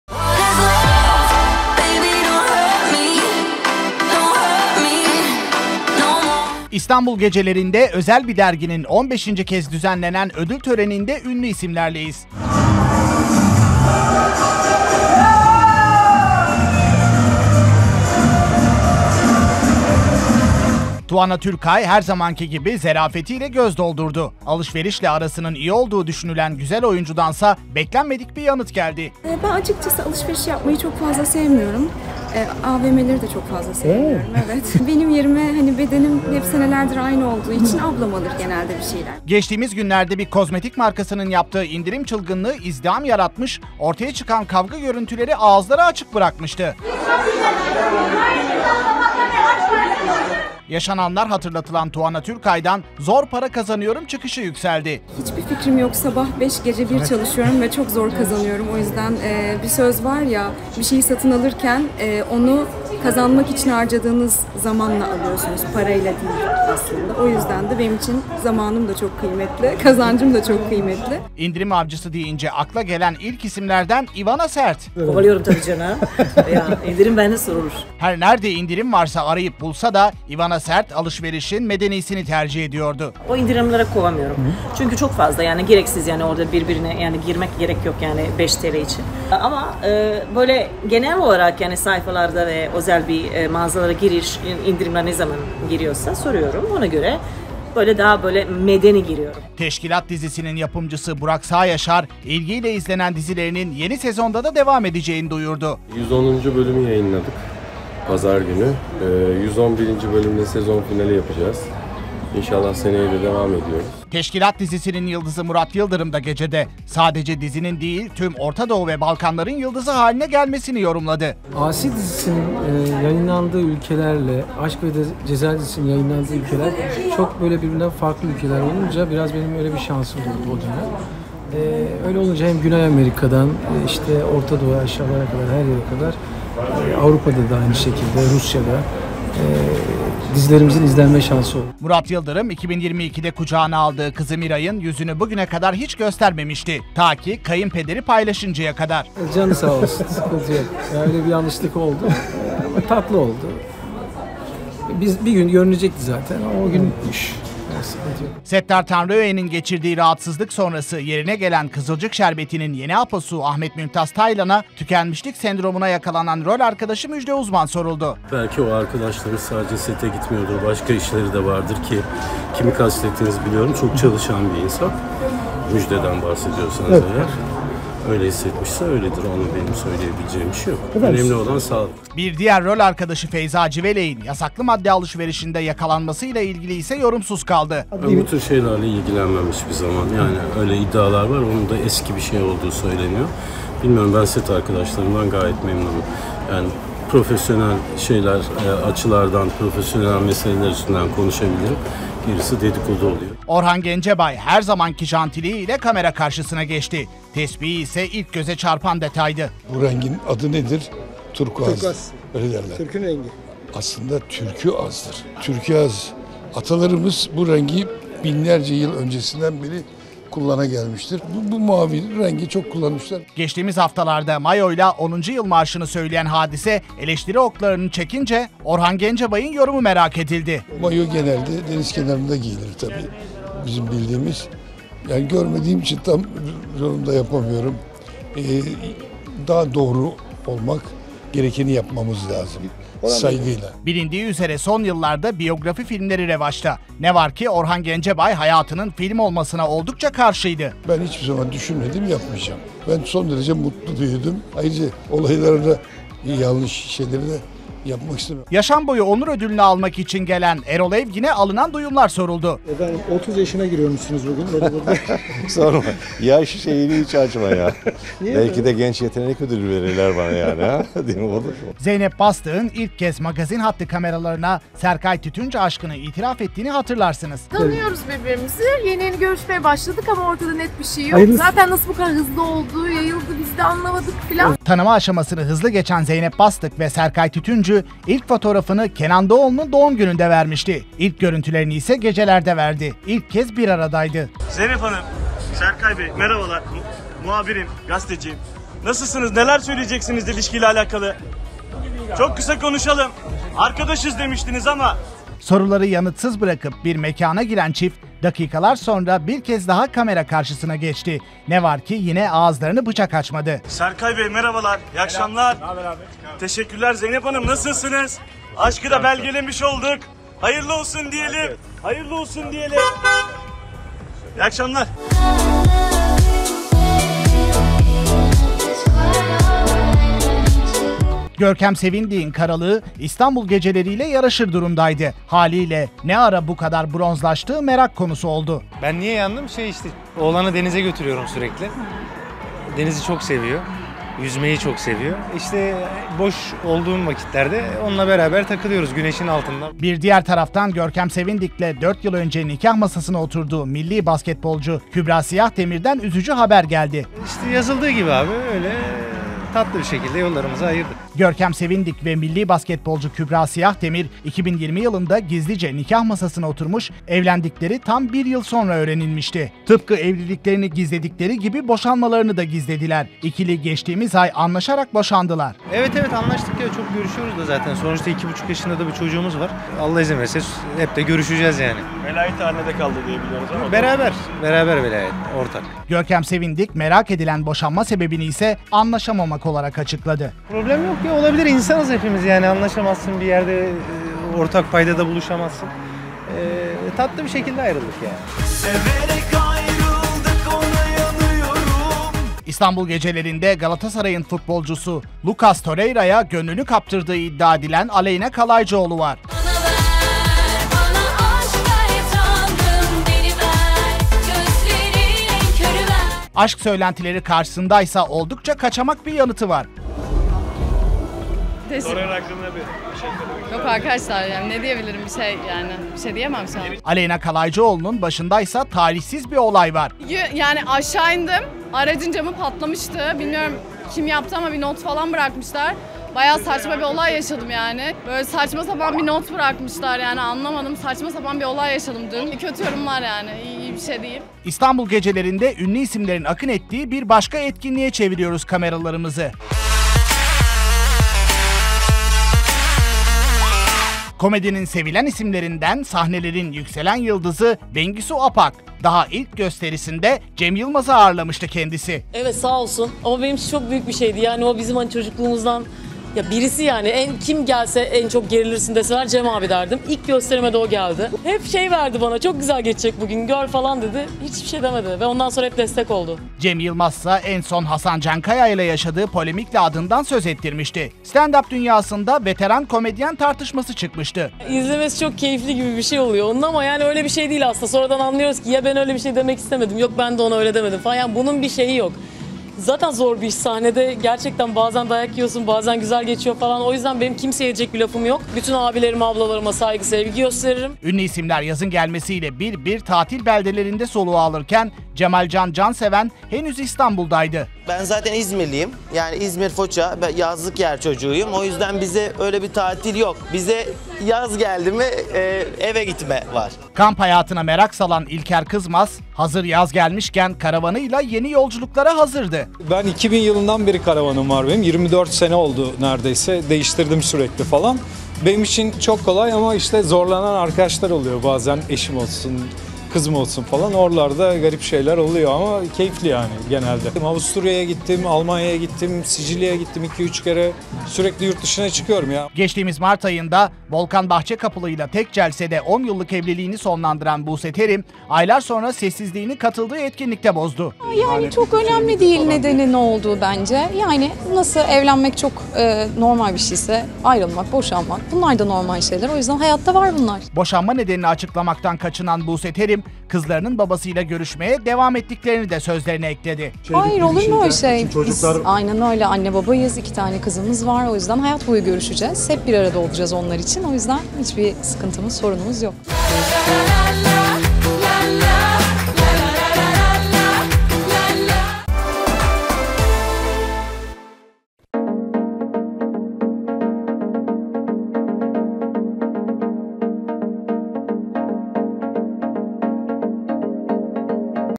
İstanbul Gecelerinde Ünlülerden Olay Yaratacak Açıklamalar!
İstanbul gecelerinde ünlülerden olay yaratacak açıklamalar!  "GECE HAYATINDA MAGAZİNE YAKALANANLAR"

TÜRKSAT 4A
Frekans: 12729
Sembol: 30000
Polarizasyon: V (Dikey)